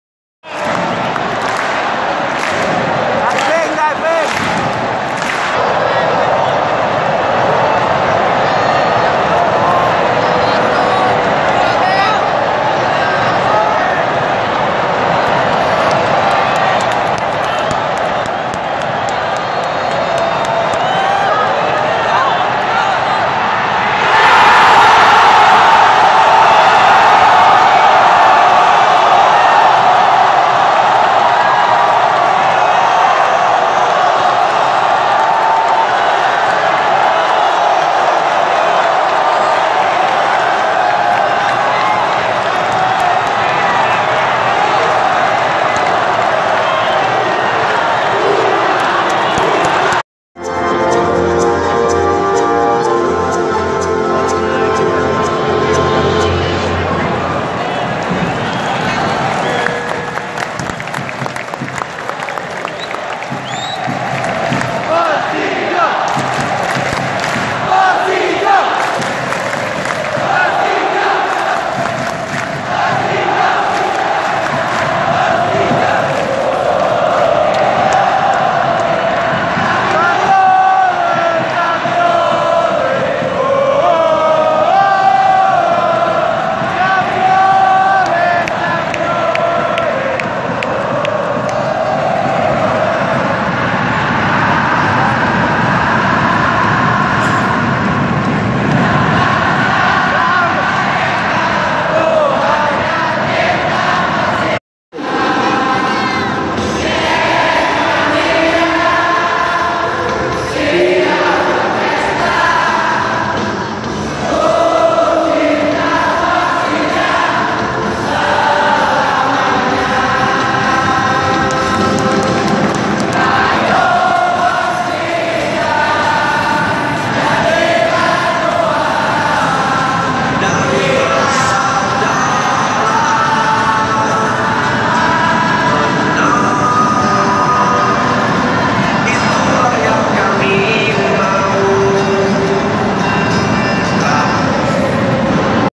Detik - Detik Goal Pertama Persija
Persija VS Mitra Kukar

2 - 1 persija unggul dengan di awali goal pertama dari Marko Simick